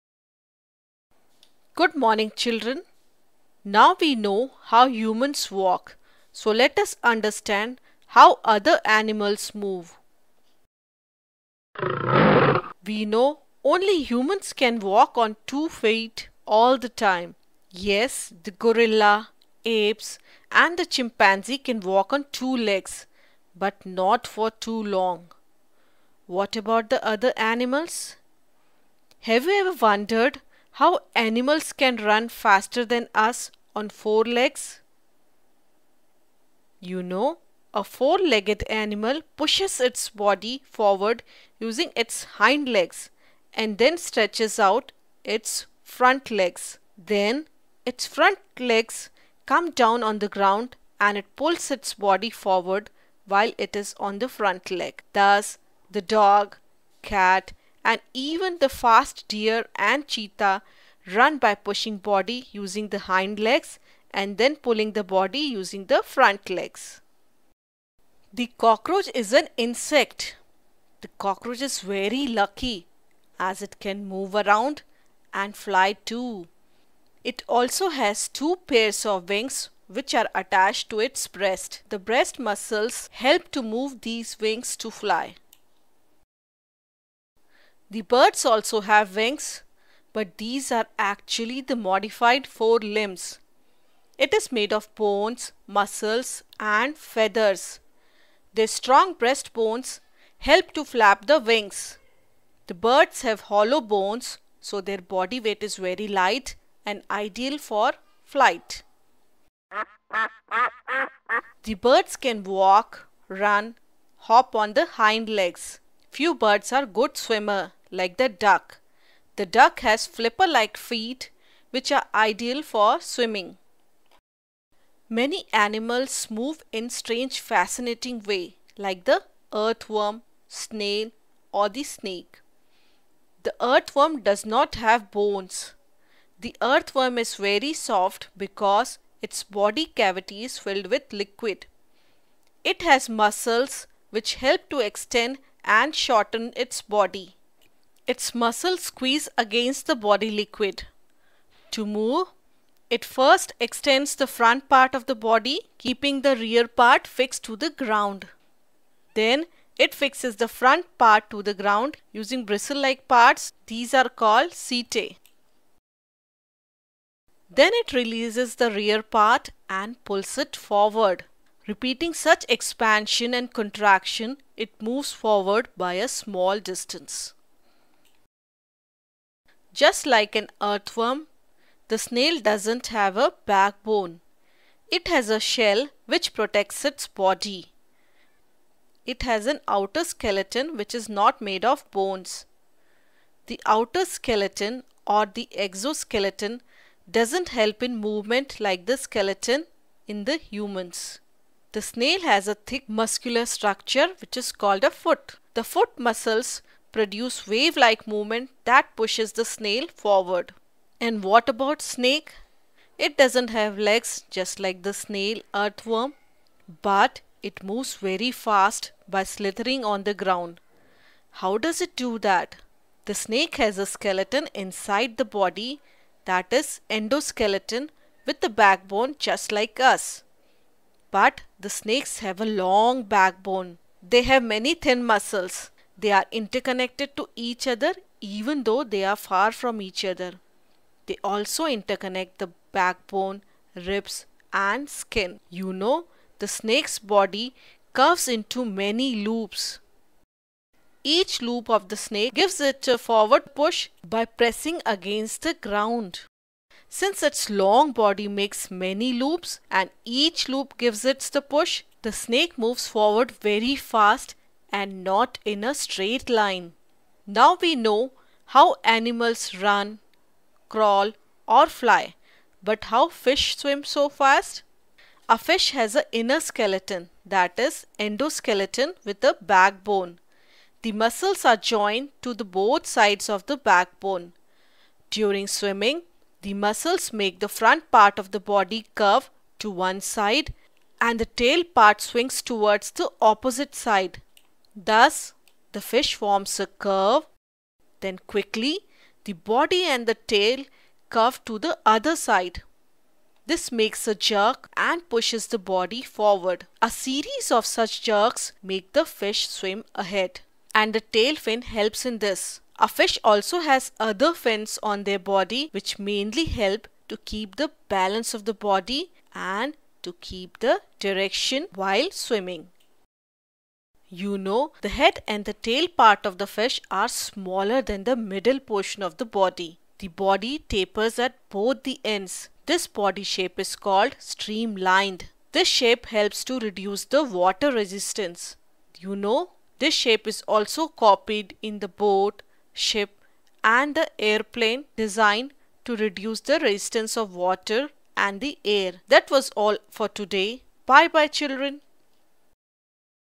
Science -- How do animals move - English
Hello,
BodhaGuru Learning proudly presents an animated video in English which explains how different animals move. It shows how earthworm, snake, snail move without leg, how lion runs so fast. It also explains how fishes swim so fast.

Team BodhaGuru